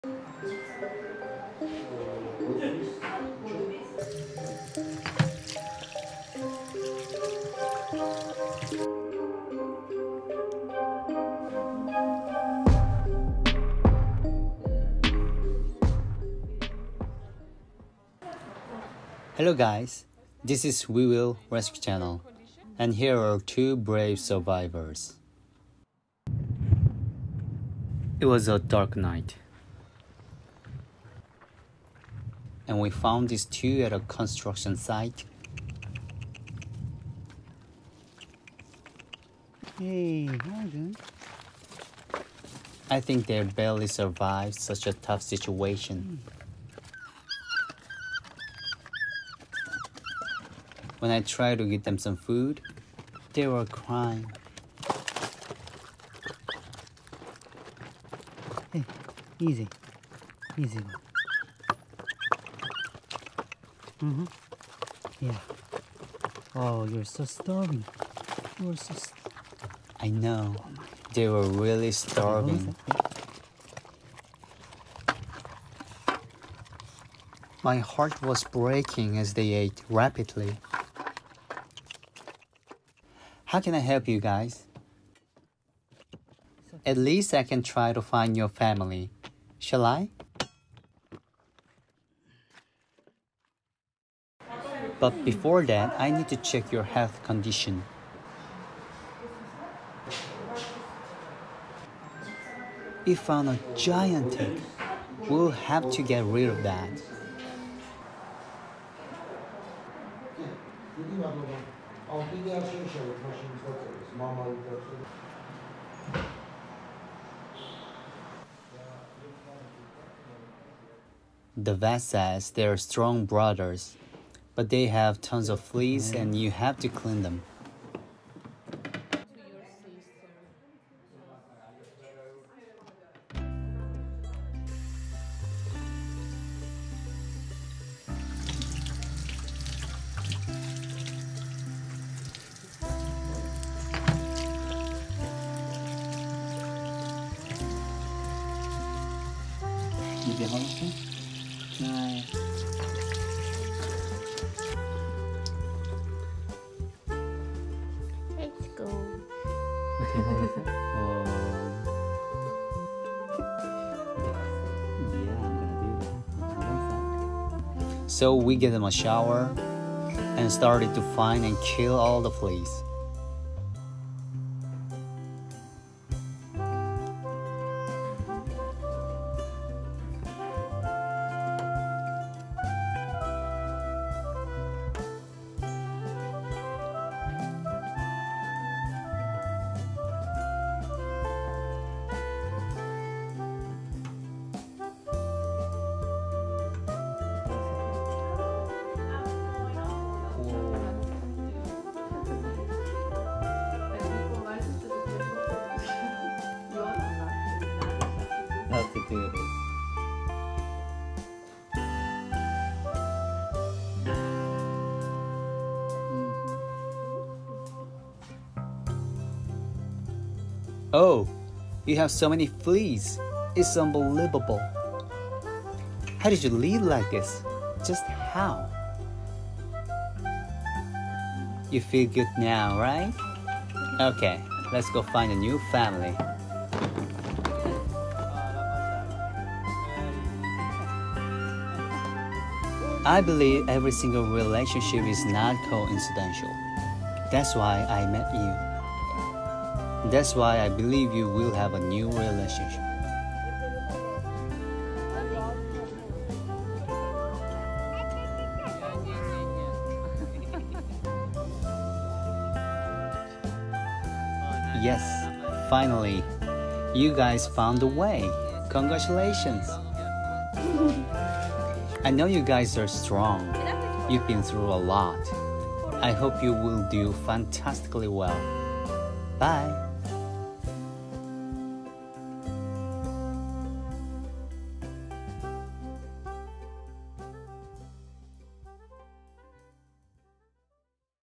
We rescued two brave puppies and gave them a fantastic shower!
This is 'We Will Rescue' Channel. We want to introduce you to two brave survivors. It was a dark night and we rescued these two puppies at a construction site. I think they barely survived such a tough situation. They have tons of fleas and gaint ticks! What happened to them?